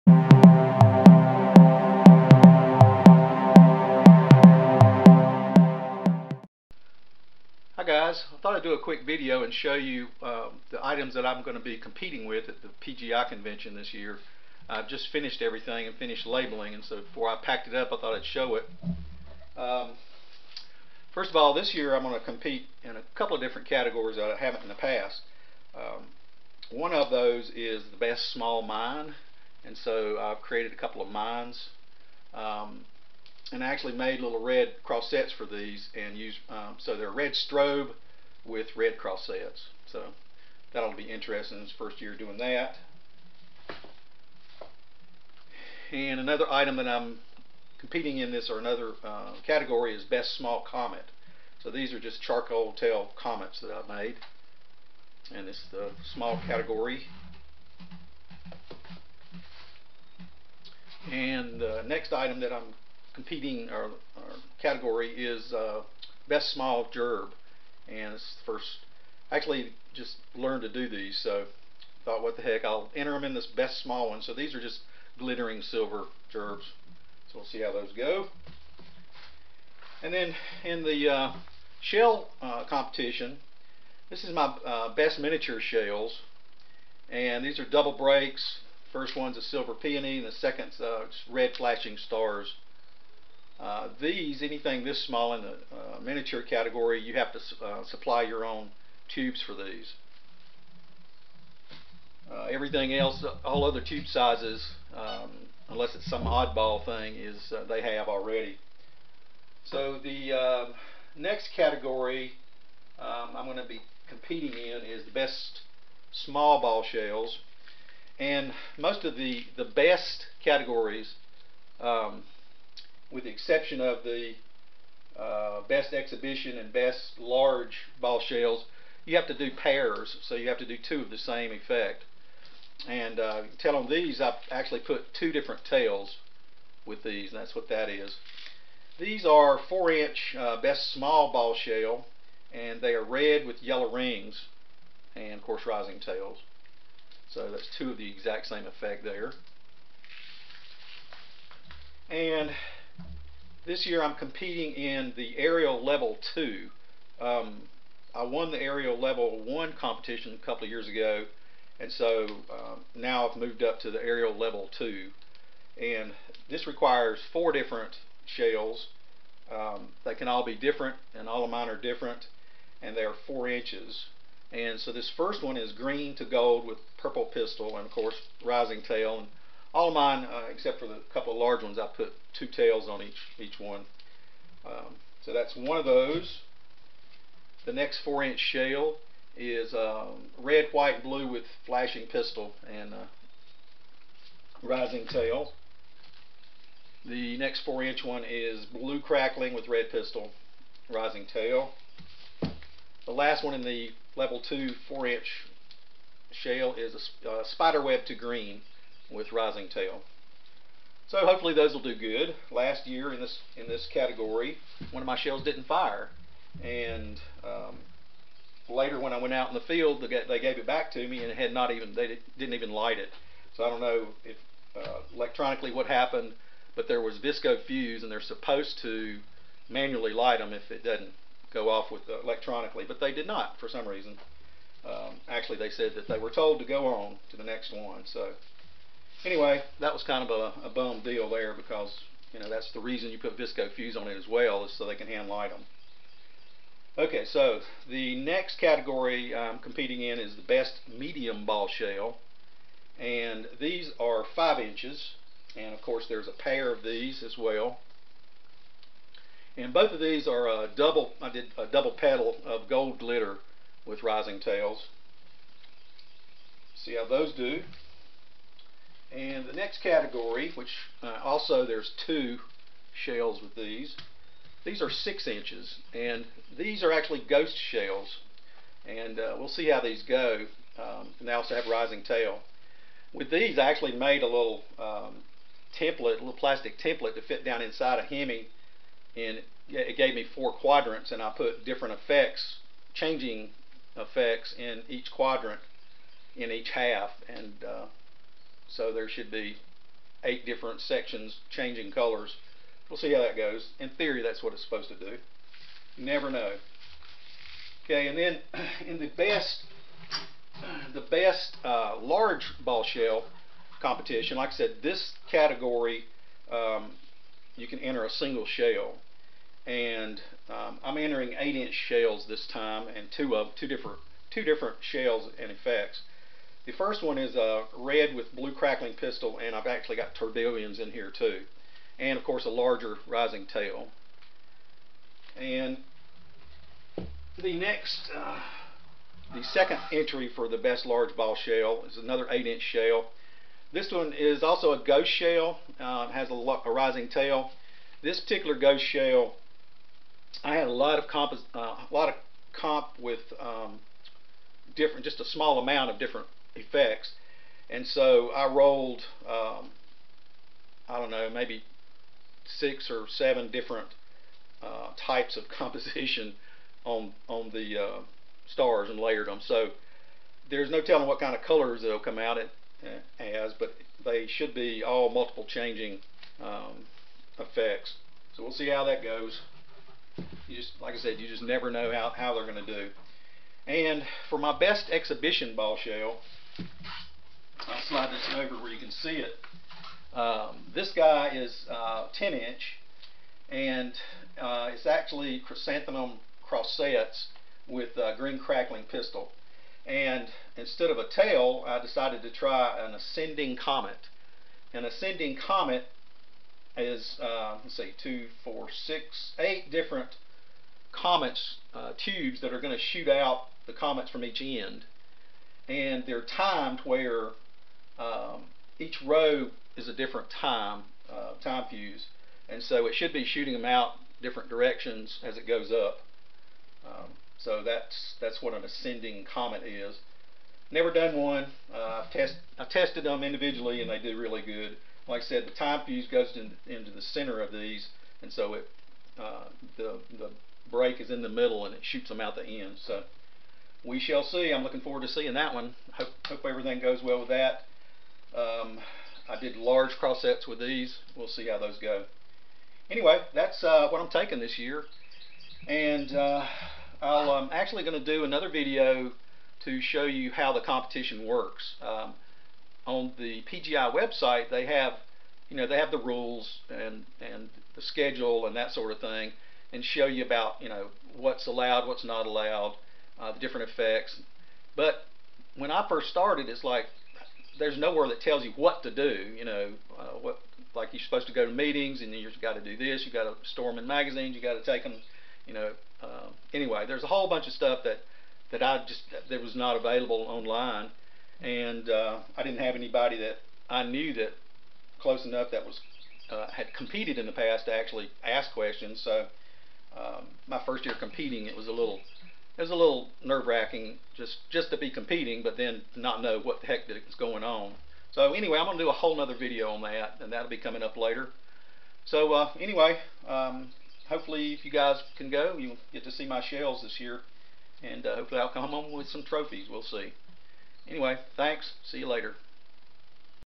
PGI Competition Shells 2019 - Tarheel Pyro Ep 18
Review of my shells, etc for the PGI competition this year in Gillette Wyoming.